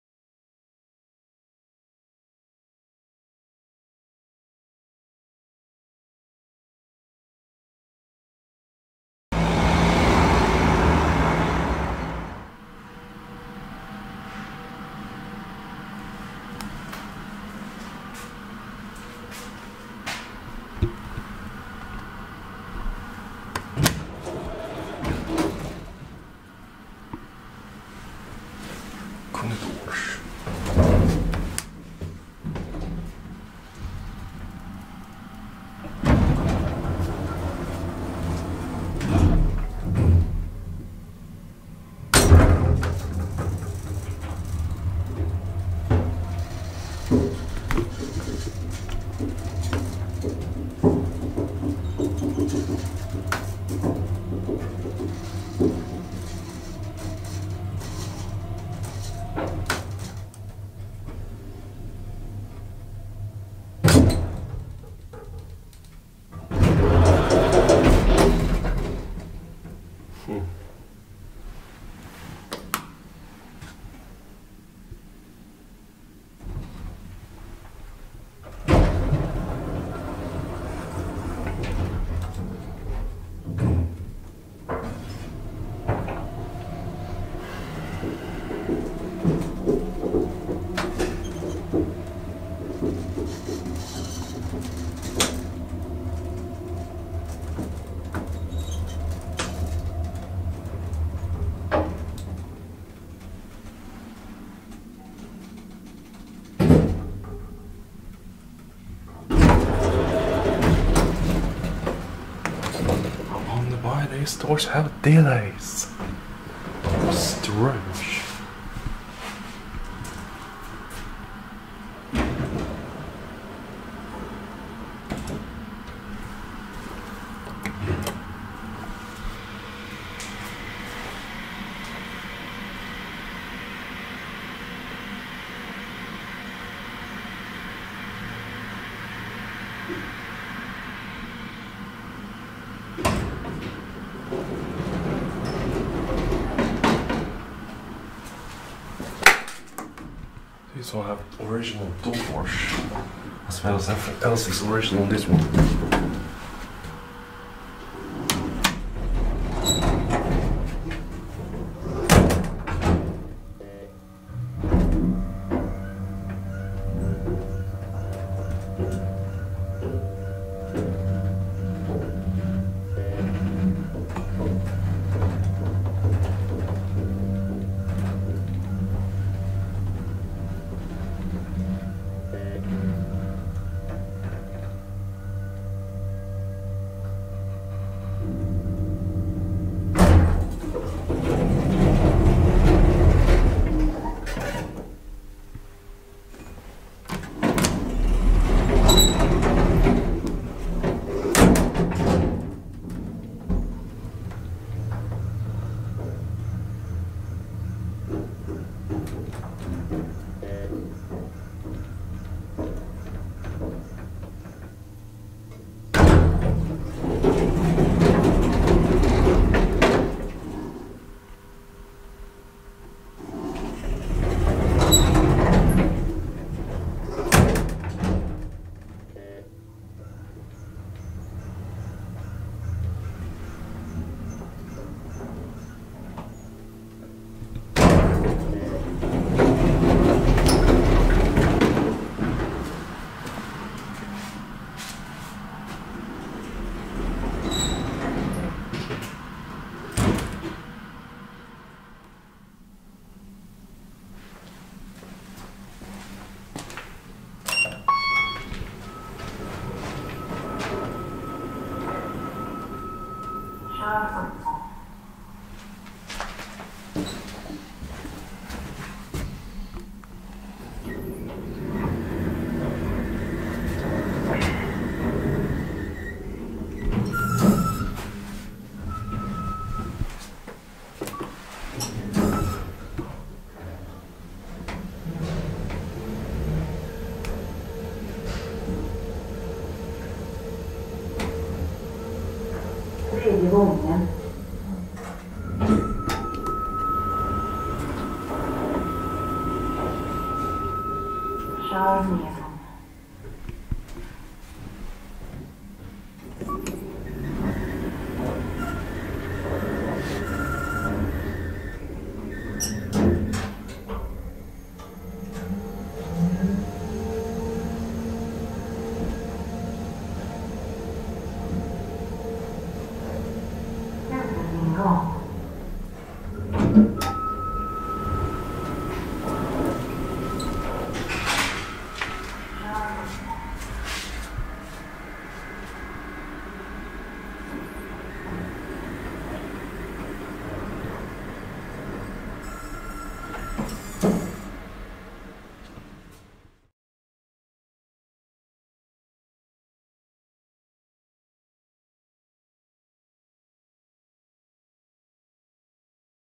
Bauer & KONE Elevators @ Tranebergsplan, Stockholm
INTRO: 0:00
#1 - Bauer mod. by KONE (10394): 0:22
#2 - Bauer Original (10393): 2:08
#3 - 2014 KONE MonoSpace (42084803): 4:12

Location: Tranebergsplan 1-3, Stockholm (Alvik) SWEDEN!